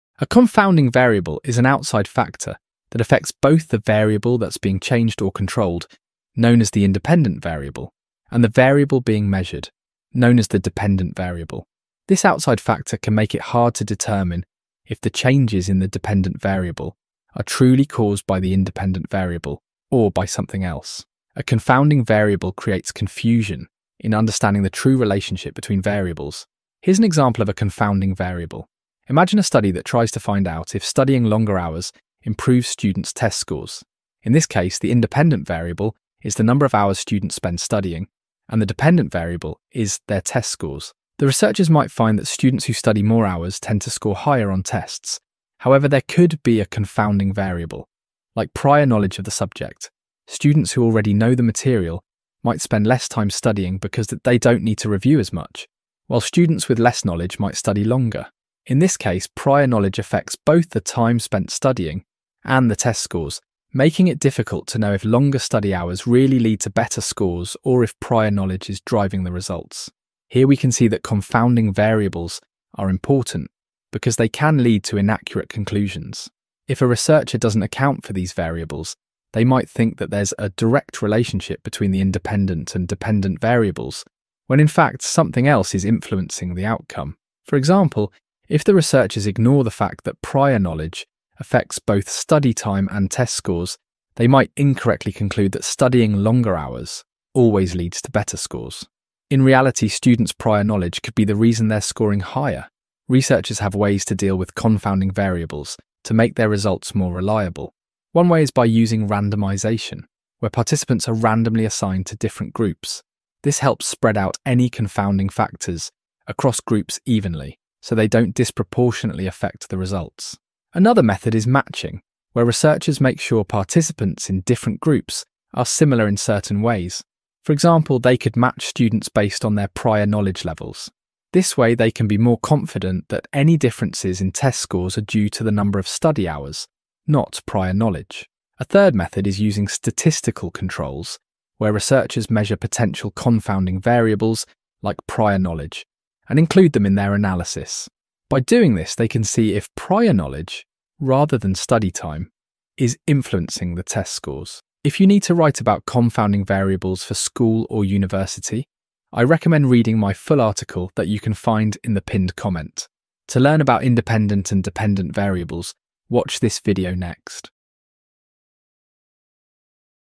Confounding Variables: Definition & Examples (3 Minute Explanation)
A confounding variable is an external factor in a study or experiment that affects both the independent and dependent variables, potentially skewing the results. It can create a false impression of the relationship between the variables being studied. Researchers aim to identify and control for confounding variables to ensure the accuracy and reliability of their findings.